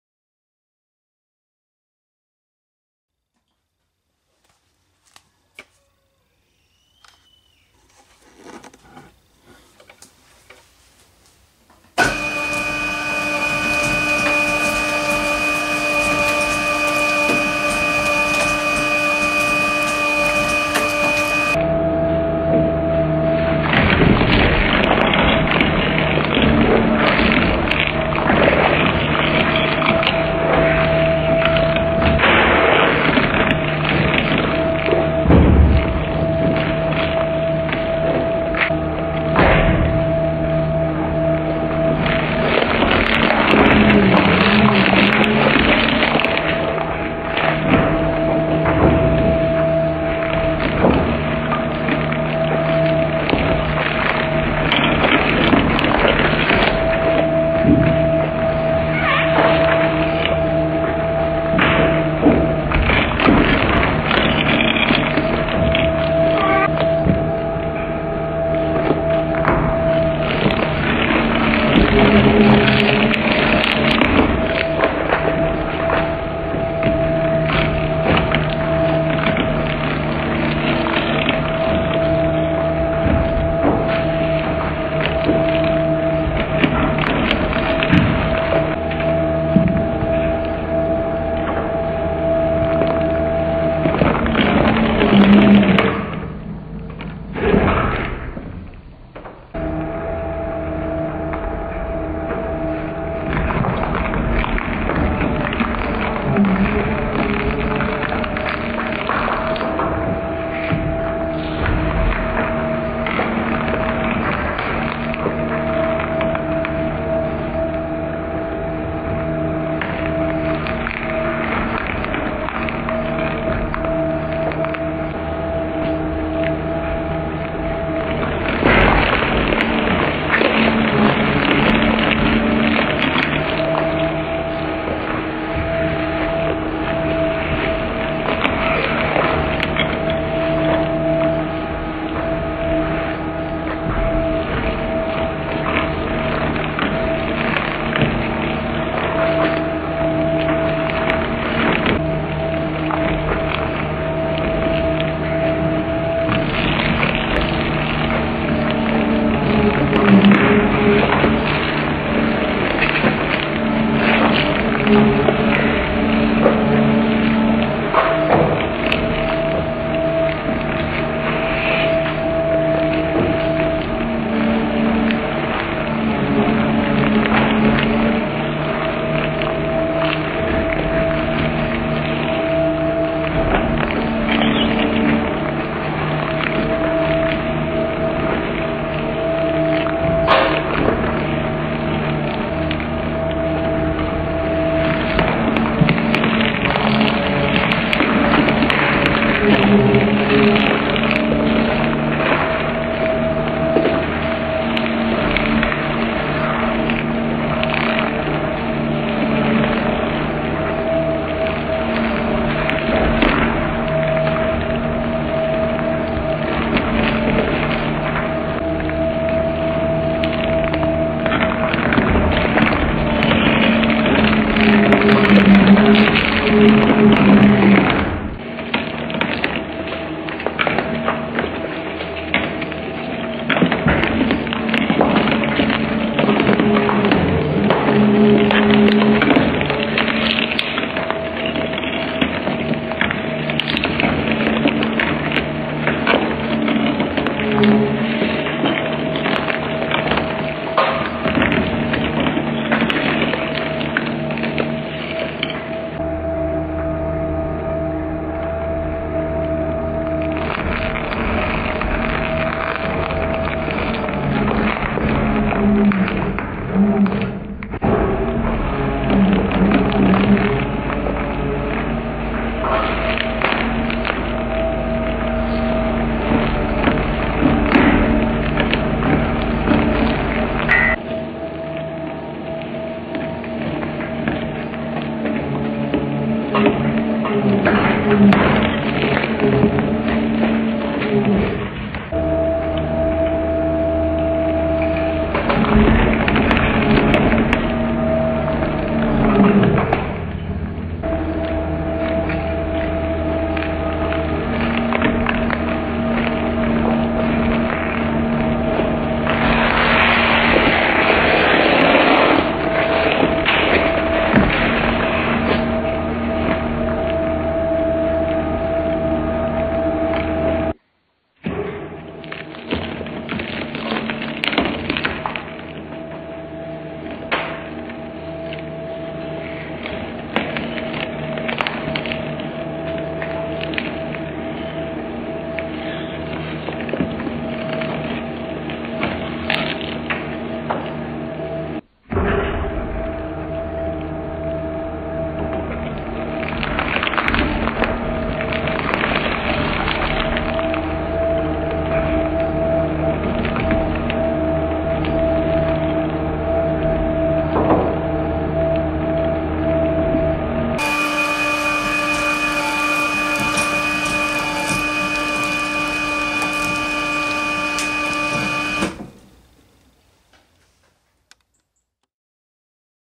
Shredder VS. Children's Shoes - Sneakerkonvolut 012 slow
Here is the slow-motion version of the compilation of the most beautiful scenes of the sweet children's shoes in the shredder.